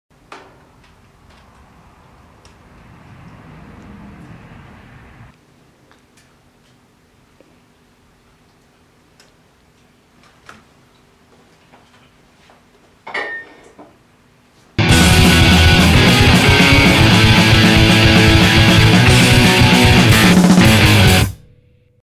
Pitt the Elder - Behind the Scenes Recording Album Two
Recording an album is hard work. It takes ages and you need to be on the ball at all times. Check out what goes on behind the scenes when we lock ourselves away to make musical magic.